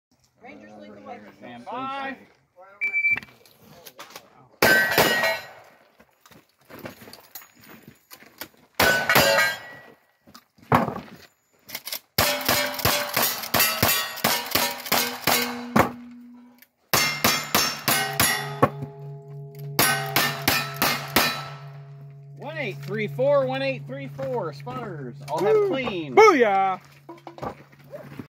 Panhandle Cowgirl - Green Mountain Regulators 22oct22
At the Green Mountain Regulators Cowboy Action Shooting club near Marble Falls, Texas